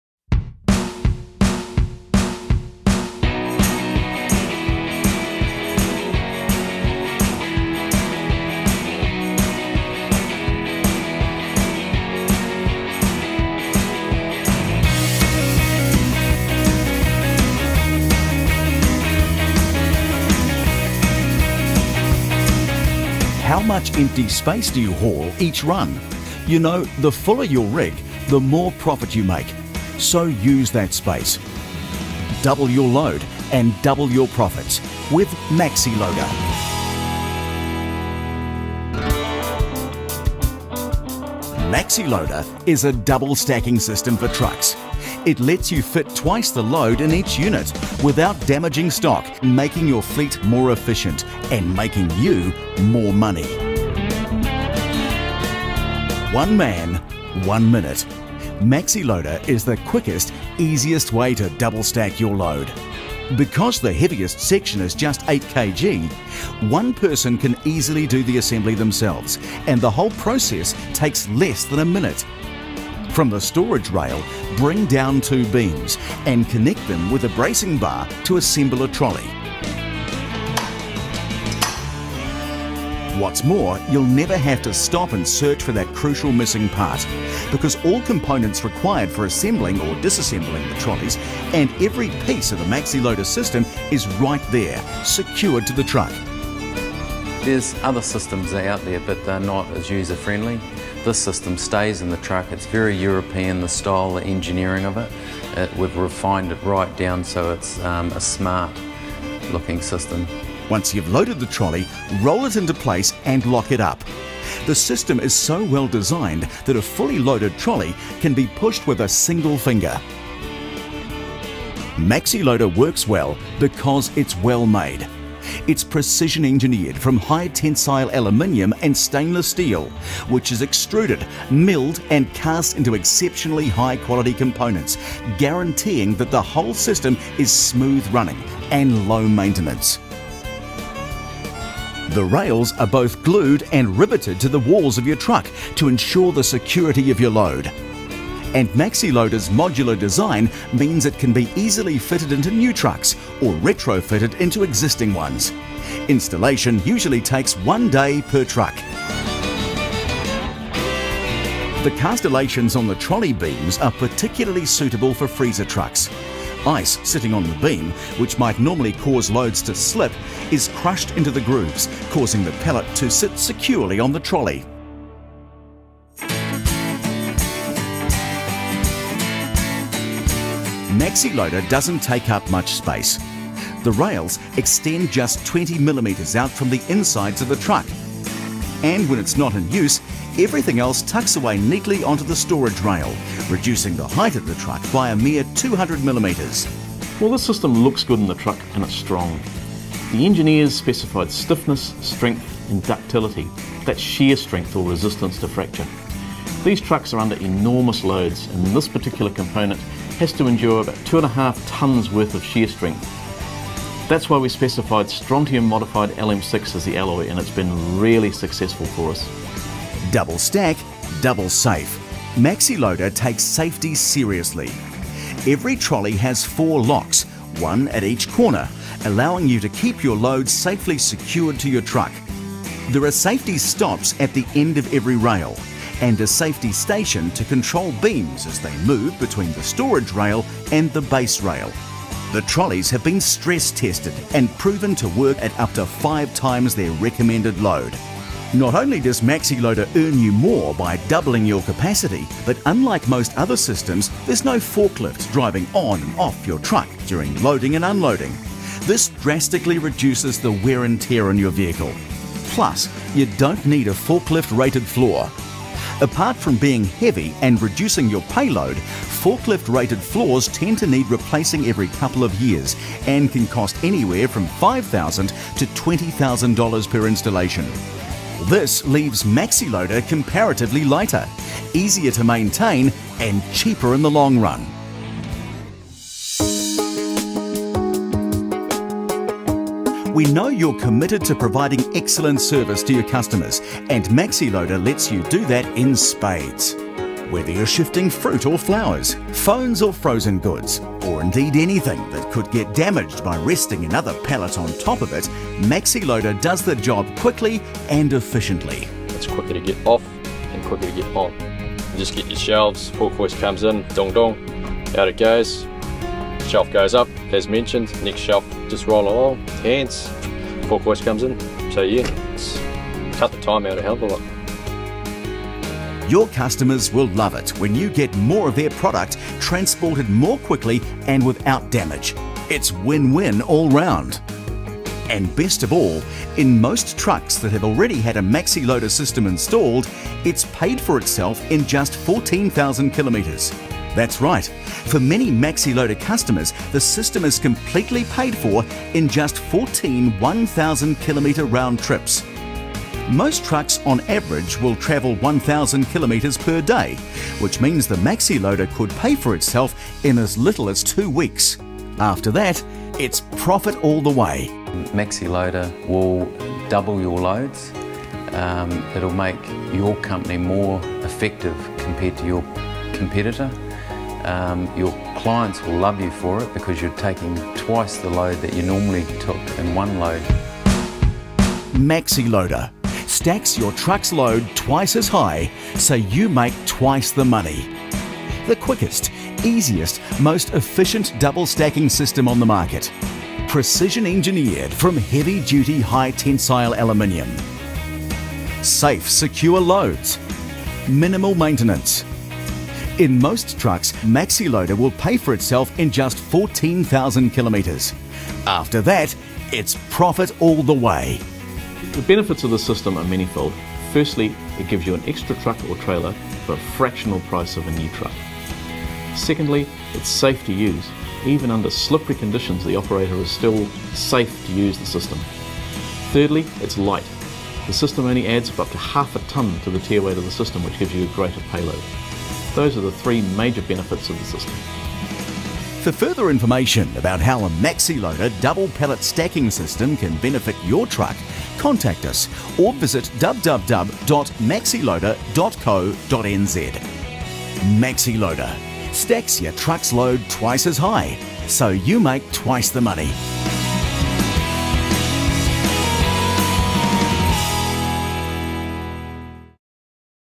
Maxiloda - Innovative Transportation Solutions!
Maxiloda - Innovative double-stacking freight systems for truck-trailer units. Safe, quick, simple, low-labour, profitable.